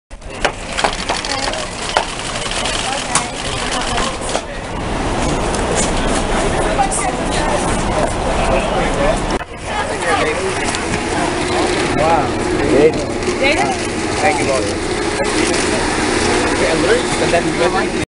Jaden Smith & Sarah Snyder in NYC (9/11/16)
Jaden is NOT 5'9!! waiting for Gigi and they come walking down the block